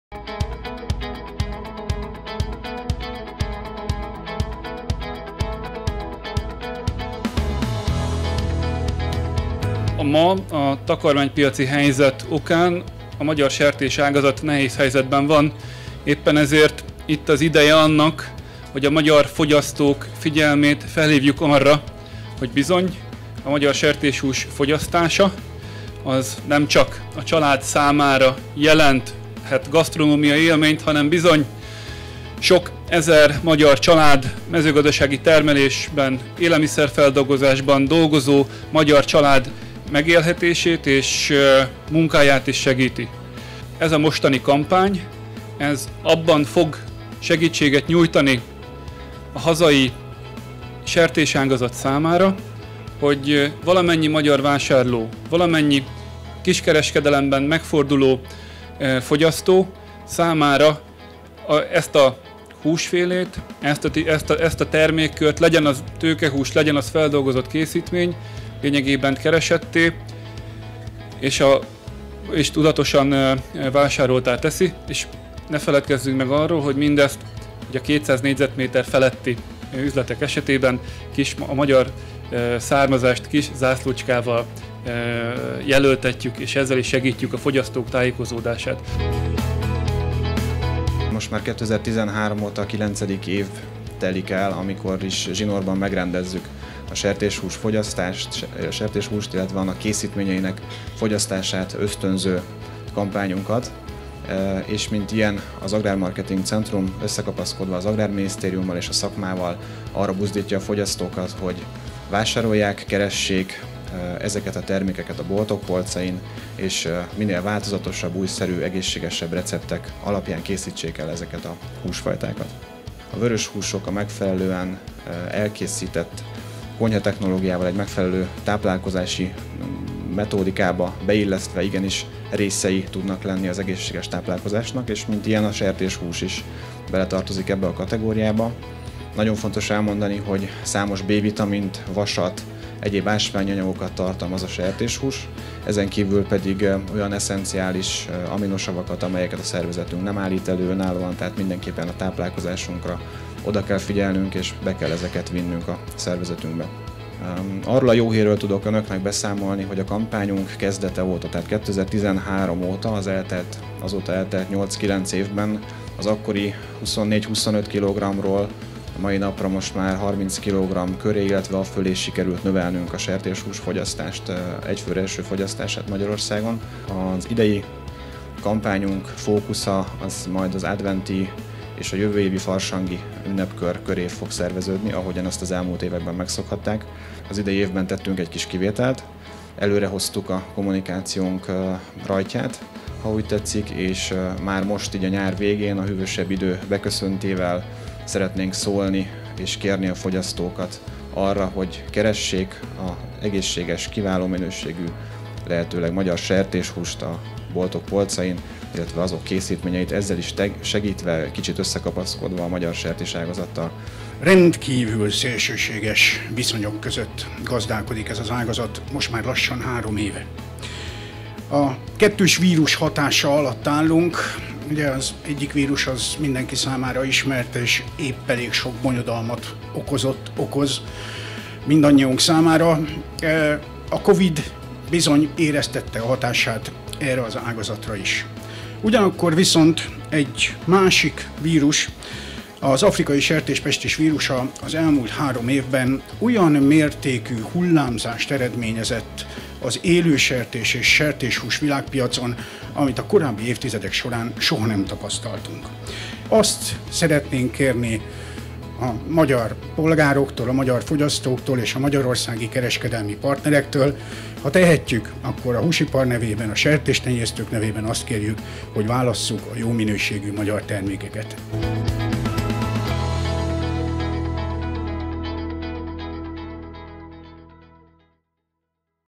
Helyzet van a sertéspiacon!
A sertéságazatot érintő nehézségek csökkentése érdekében indít fogyasztásösztönző kampányt az Agrárminisztérium, az Agrármarketing Centrum és a Magyar Húsiparosok Szövetsége.
Az alábbi videóból megtudhatod, mivel támogathatod te is a magyar gazdákat!